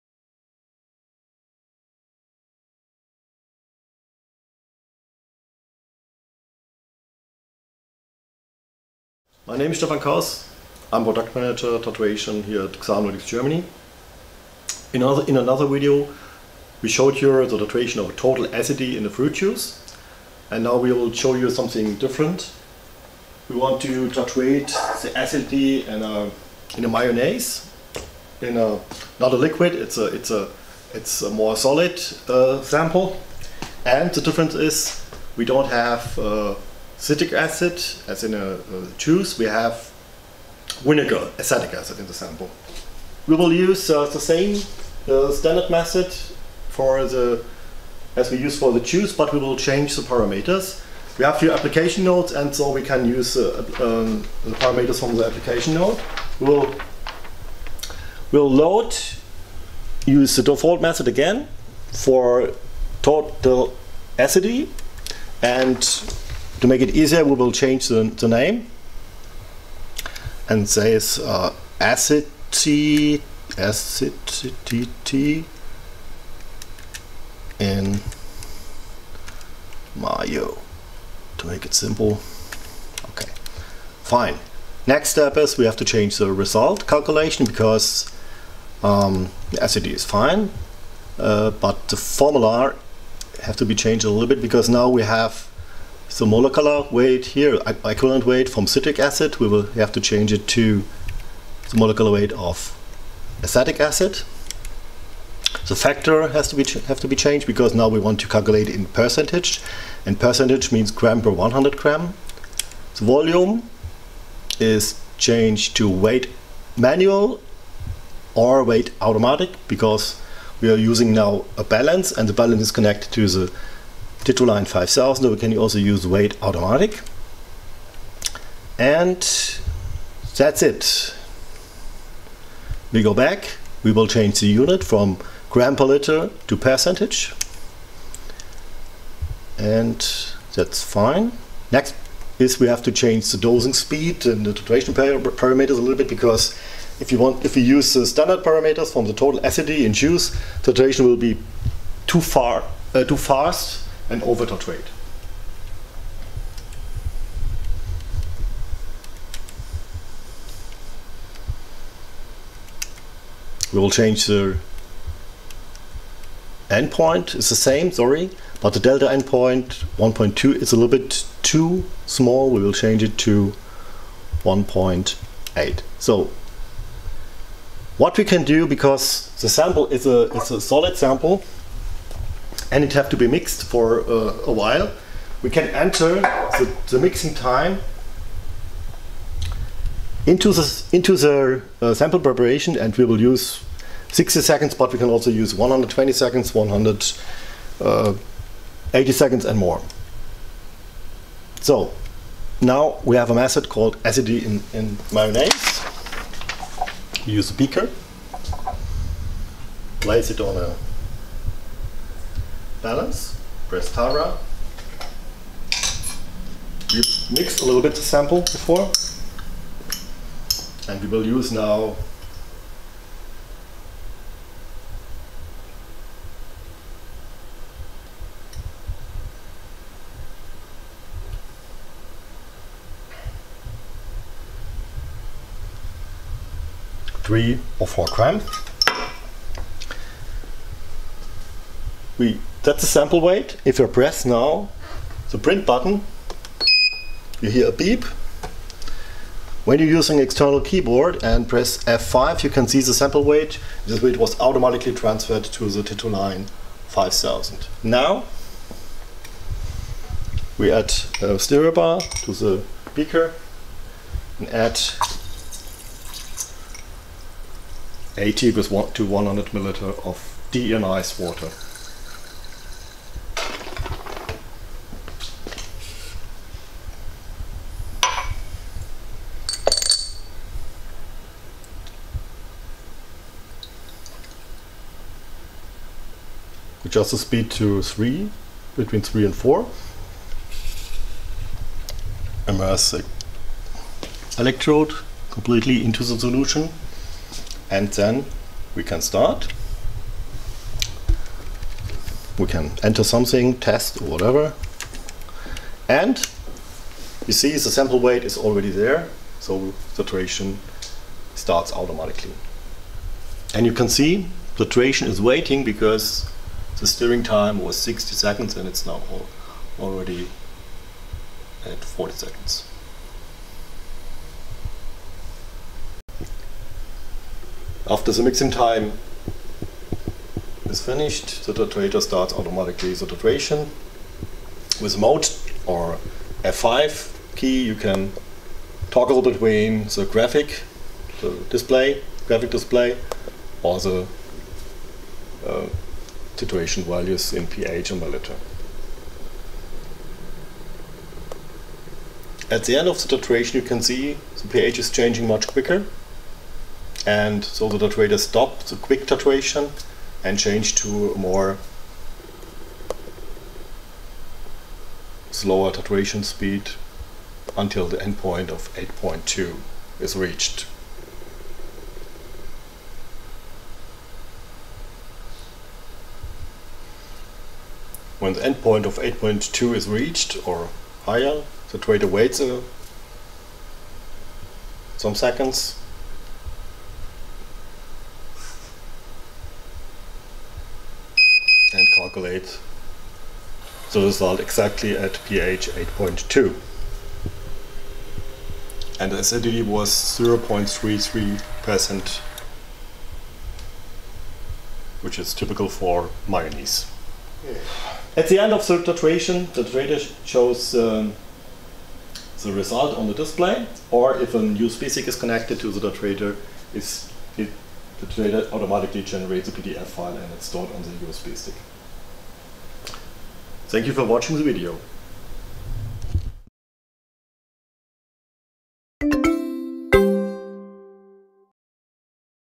Titration of Mayonnaise - TitroLine 5000 - SI Analytics, a Xylem brand
In this Video Stefan Kaus, our Product Manager for Titration, shows you how to operate a titration of mayonnaise with the SI Analytics Titrator TitroLine 5000. Please find more information about the titrator SI Analytics Titrator TitroLine® 5000